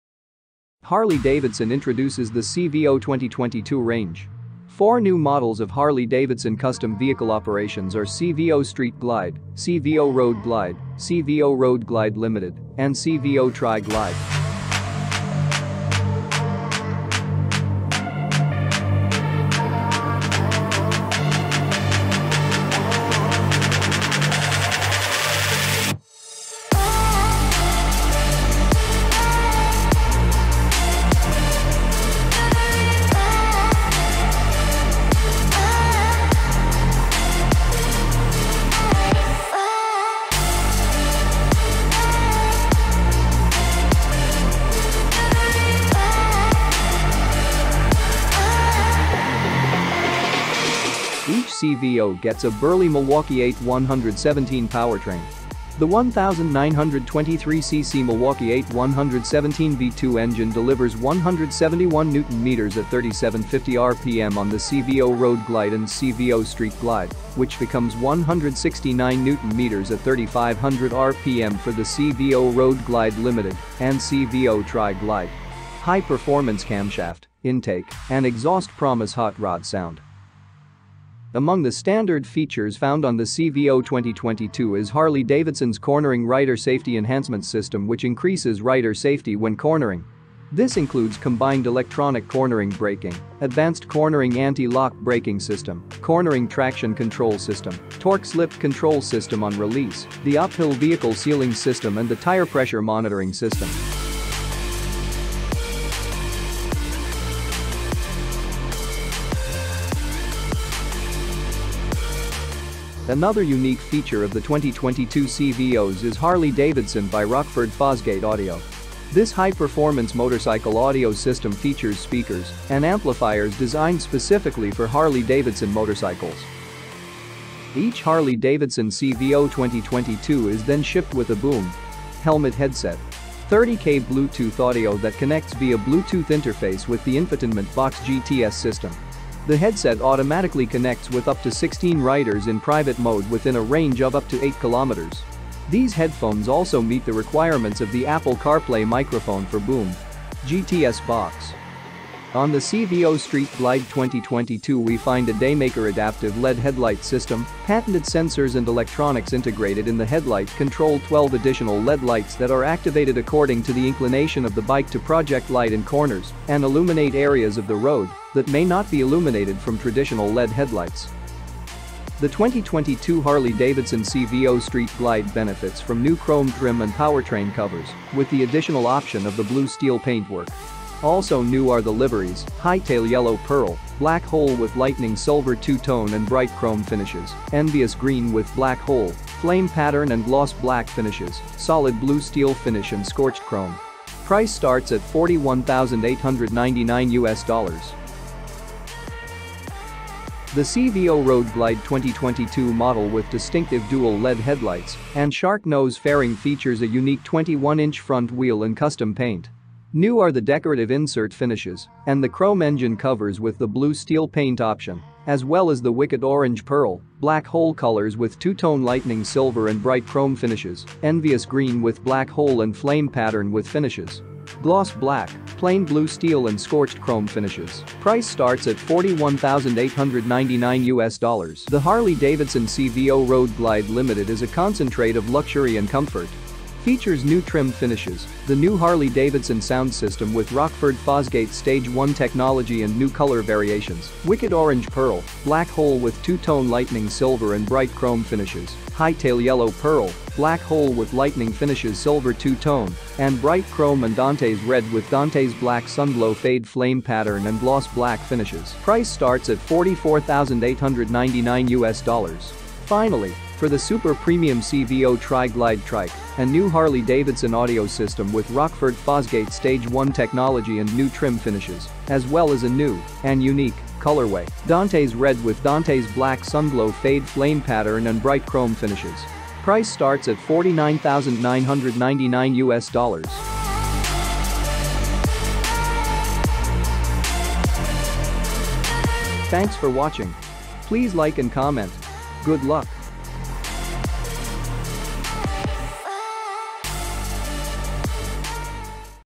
Harley-Davidson CVO Lineup 2022 - Street Glide, Road Glide, Road Glide Limited & Tri Glide
Kings of the road! Harley-Davidson introduces the CVO 2022 range. Enjoy!